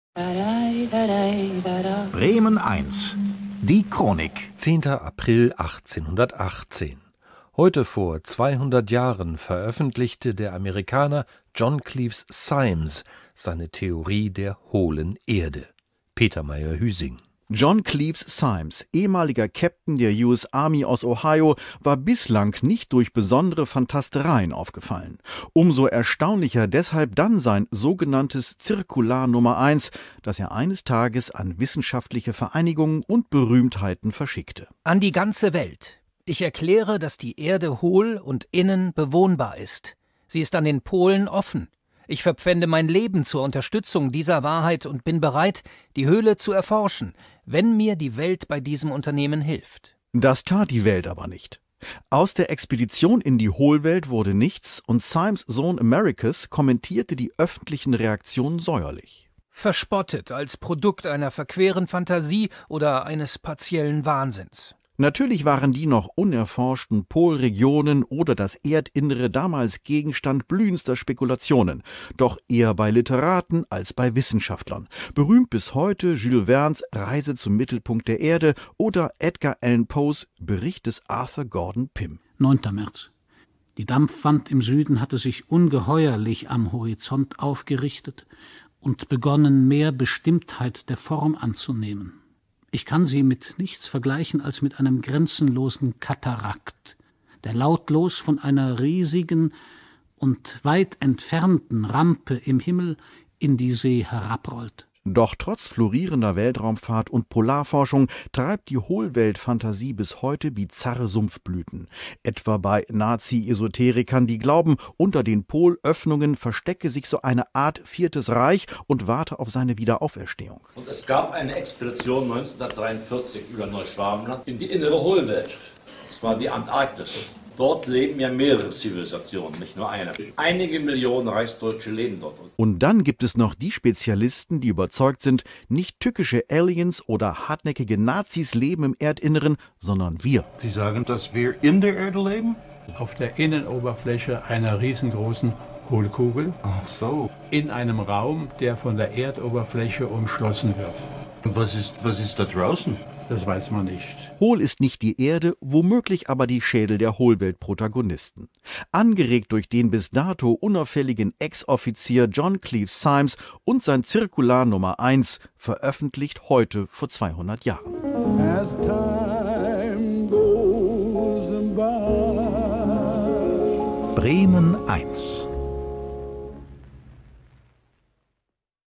10.4.1818: Eine Theorie der "Hohlen Erde"
Heute vor 200 Jahren veröffentlichte der Amerikaner John Cleves Symmes seine "Theorie der hohlen Erde".
Autor/-in:Peter Meier-Hüsing